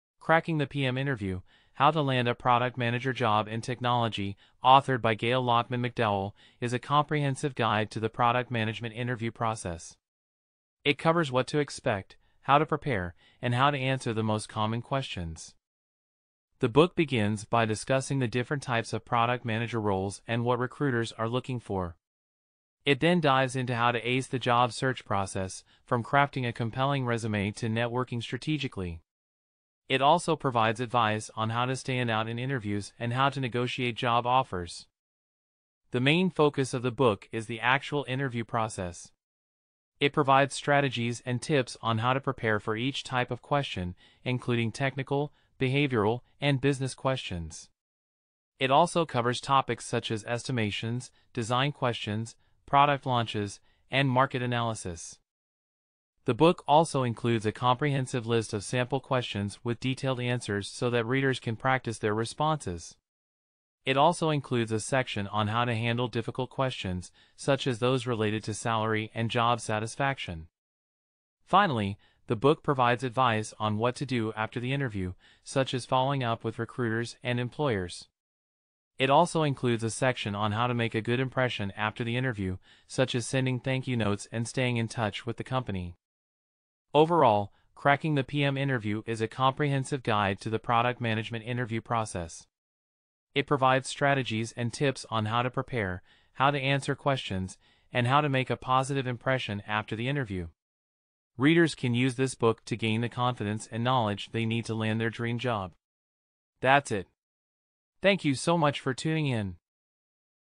Cracking the PM Interview by Gayle Laakmann McDowell
I love coffee! In this video, I'm giving a two minute summary of the book Cracking the PM Interview: How to Land a Product Manager Job in Technology by Gayle Laakmann McDowell. This book is an invaluable resource for anyone looking to break into the tech world as a product manager. Gayle Laakmann McDowell shares her extensive experience as a software engineer and hiring manager to help you understand what companies are looking for and how you can stand out from the competition. She offers advice on everything from how to structure your résumé to how to answer tough technical interview questions. So if you're looking to break into the tech world as a product manager, be sure to check out this book!